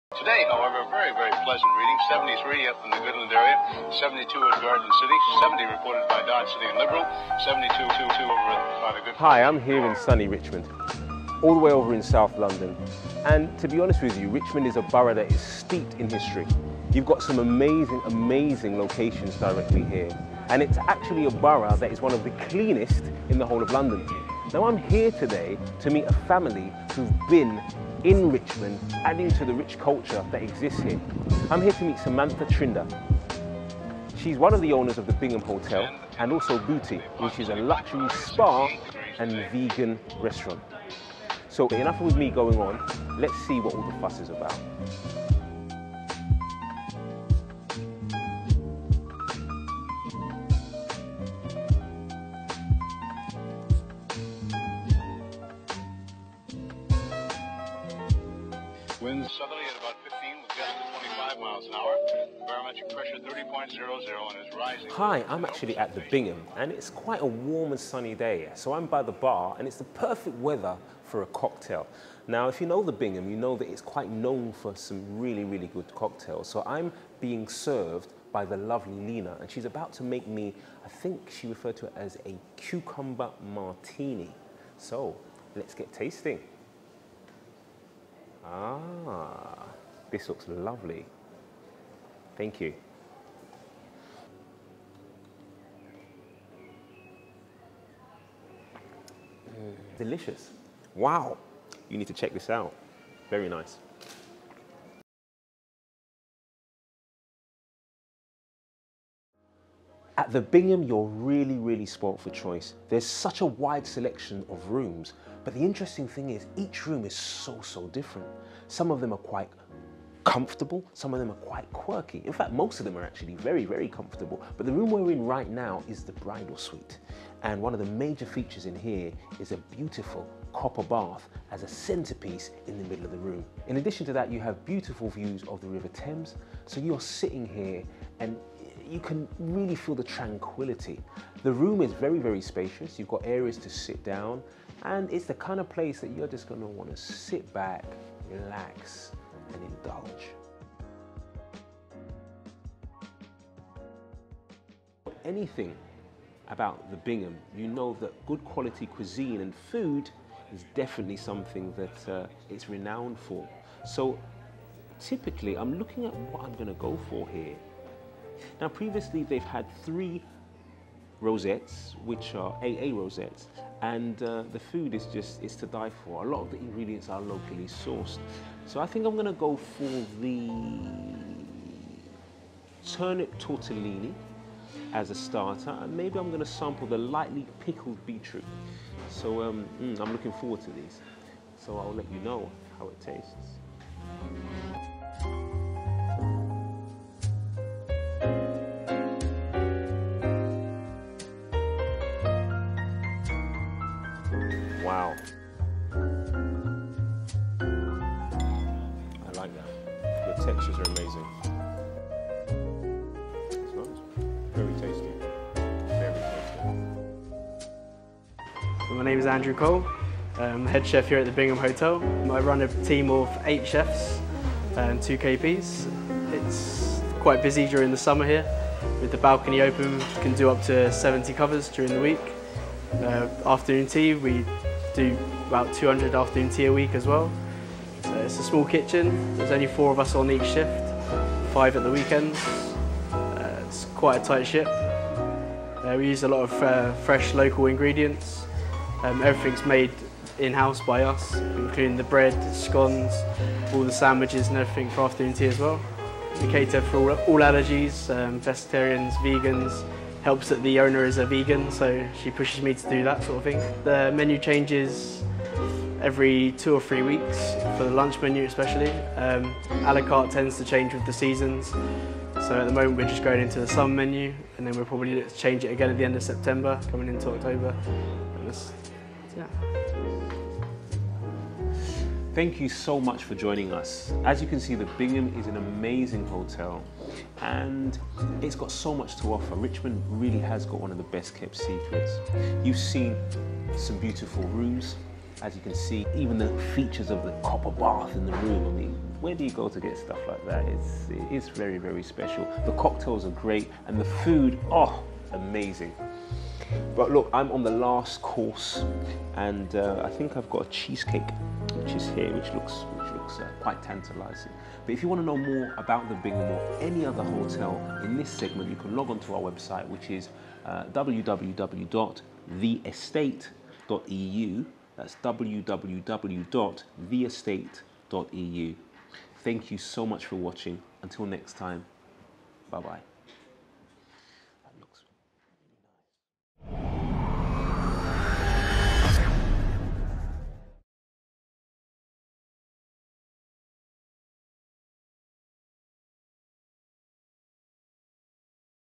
Understated and Under Rated - Best in London
The Top 5 The Bingham Hotel -

Join Joseph Farodoye as he guides you to some of London best understated Restaurants Bars and Hotels and if you like what you see use my special discount code and get a 5% discount on your order, if you are interested in getting your brand featured contact

Featured Joseph Farodoye - Host
Samantha Trinder Kenyan owner of the Bingham Hotel
Head Chef Andrew Cole

Instagram
Facebook